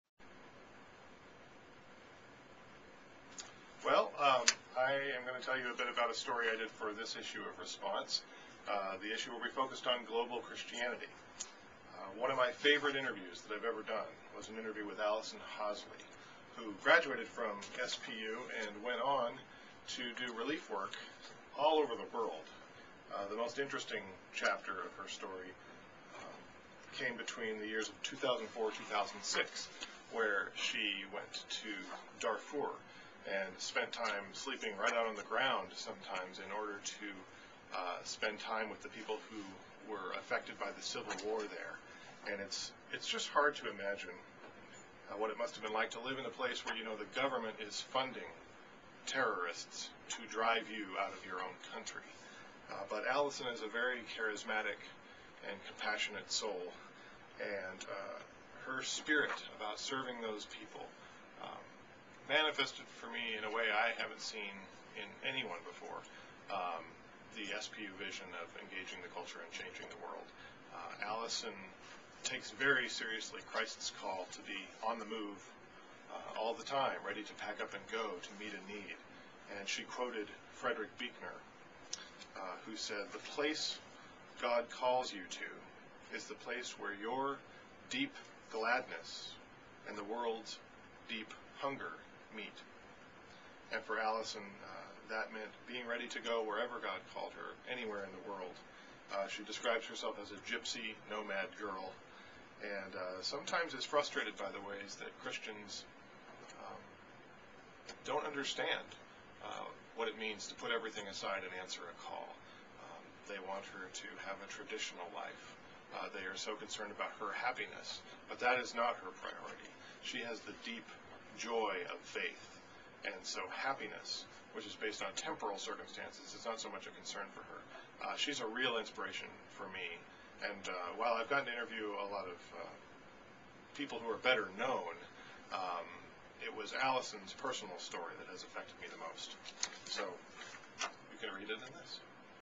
Response writer Jeffrey Overstreet
Jeffrey Overstreet tells about his most memorable Response magazine interview, and why this alumna's radical faith changed him.